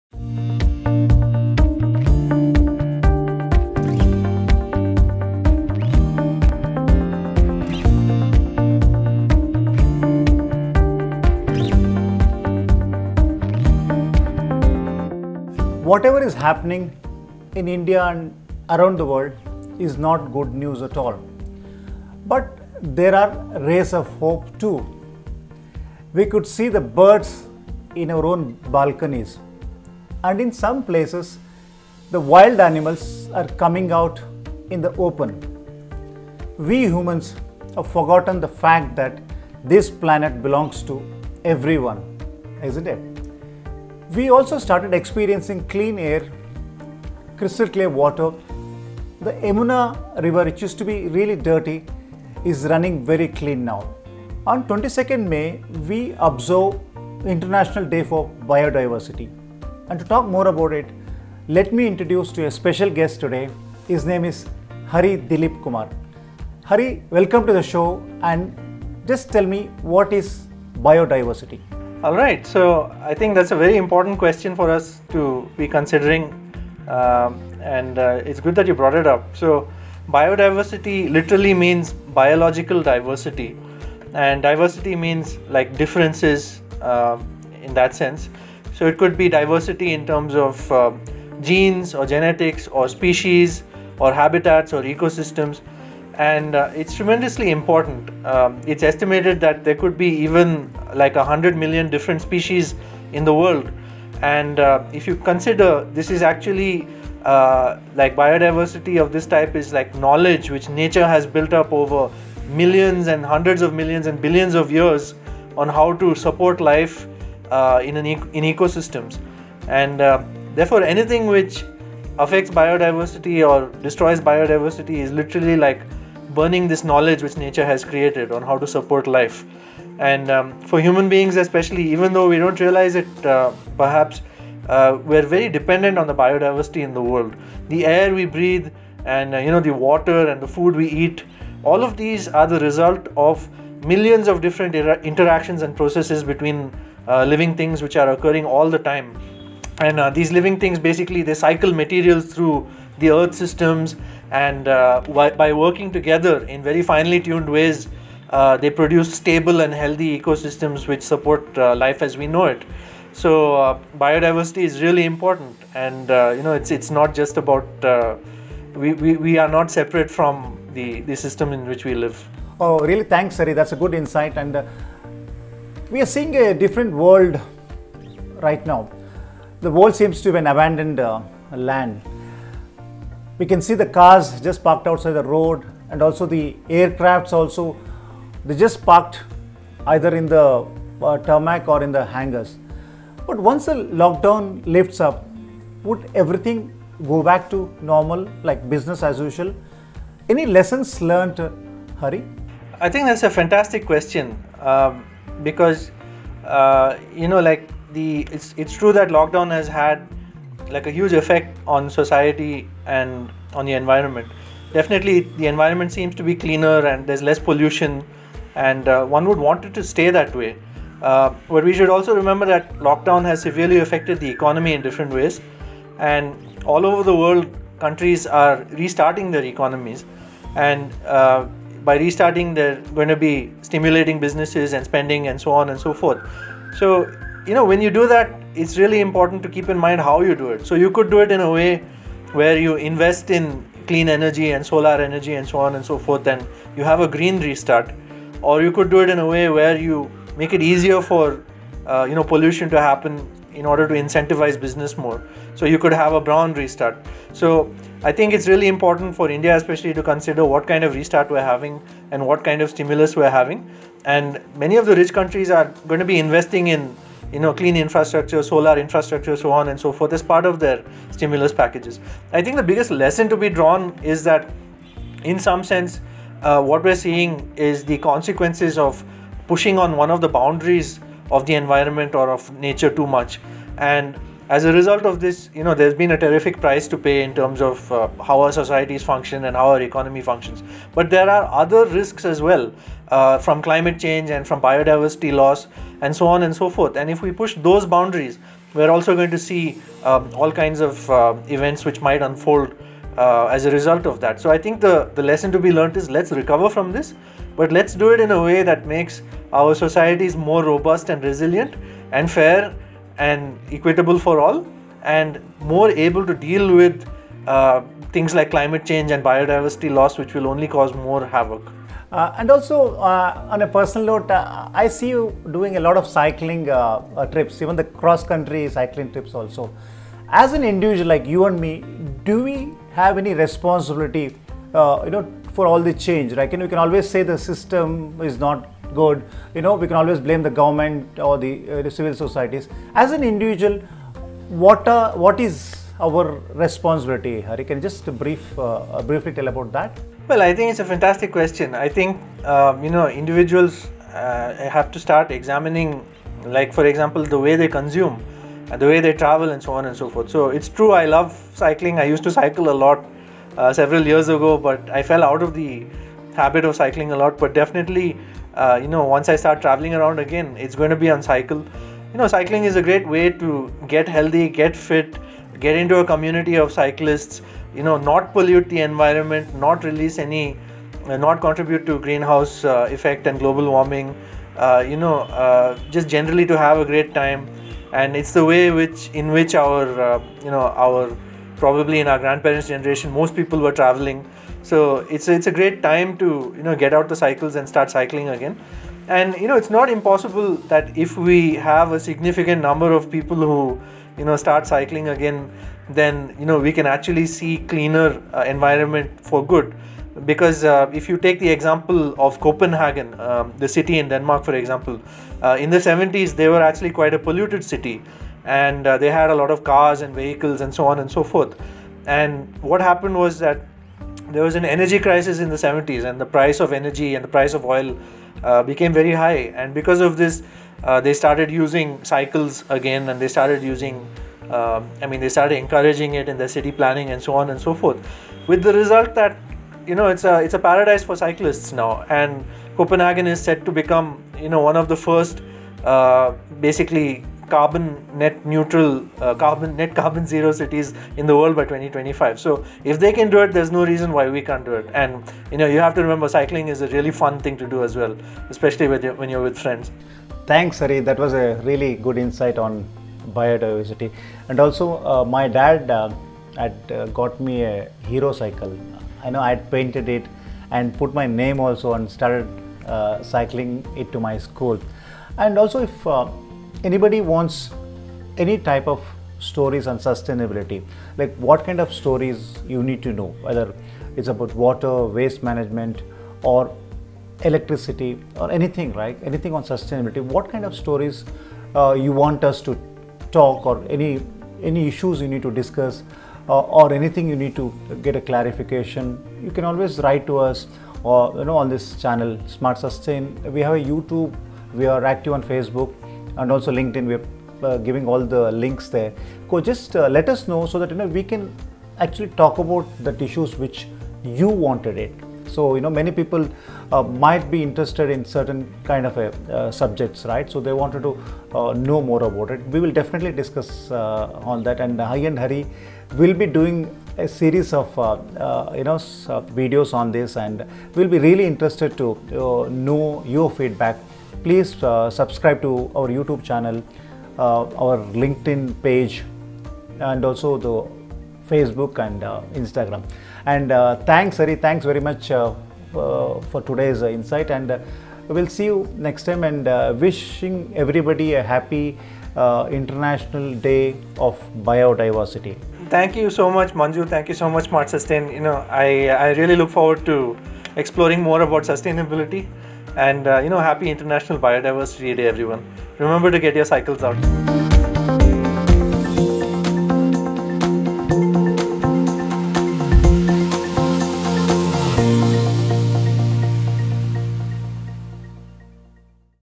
The International Day for Biological Diversity- Hari Dilip Kumar is talking to SmartSustain
The United Nations has declared May 22 as The International Day for Biological Diversity to increase the understanding and the awareness of biodiversity issues we face.
Smart Sustain has a special guest on this day, Hari Dilip Kumar, who discussed the various aspects of biodiversity and the hazards of losing it, with Manjunath.